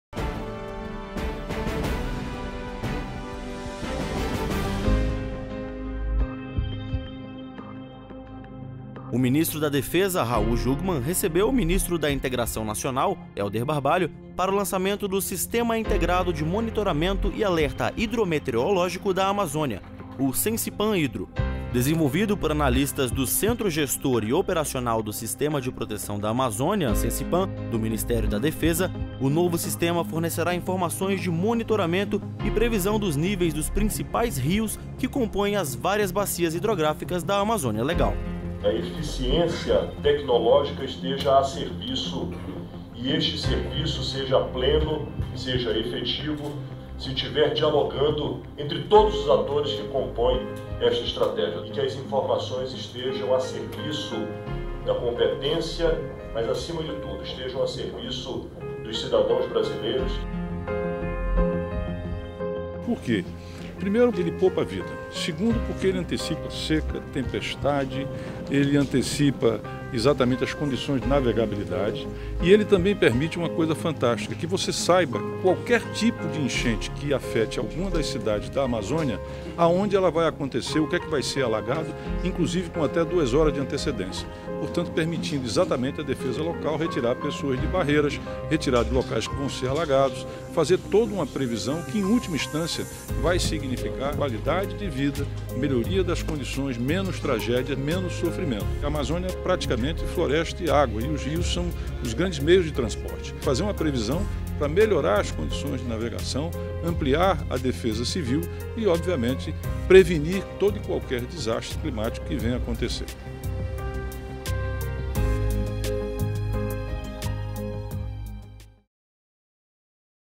TV Defesa: Novo sistema permite avisos em casos de seca e enchentes na região Amazônica
Brasília, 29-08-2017 - O ministro da Defesa, Raul Jungmann, recebeu o ministro da Integração Nacional, Helder Barbalho, para o lançamento do Sistema Integrado de Monitoramento e Alerta Hidrometeorológico da Amazônia, o SIPAMhidro. Ele permite monitorar os níveis dos principais rios que compõem as bacias da Amazônia Legal.